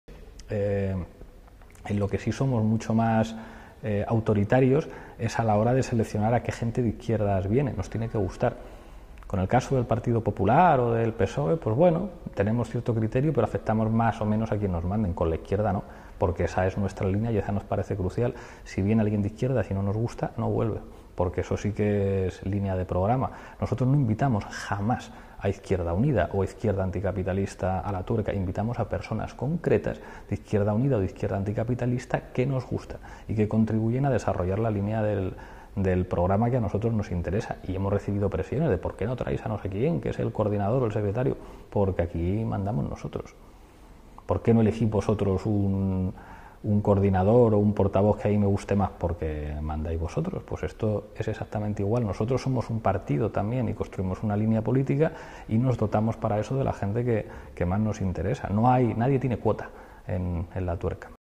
Pablo Iglesias y los tertulianos
Declaraciones hechas por Pablo Iglesias en noviembre de 2013 a Galiza Ano Cero, reservándose el derecho a invitar a su programa "La Tuerka" a los tertulianos que él decida.